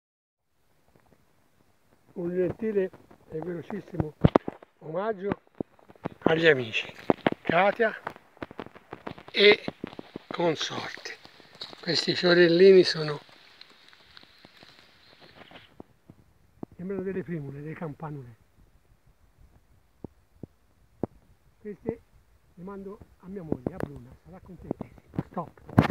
1° COLORI dei FIORI MONTANI
un meraviglioso mare di verde smeraldo siamo all'abetone con amici!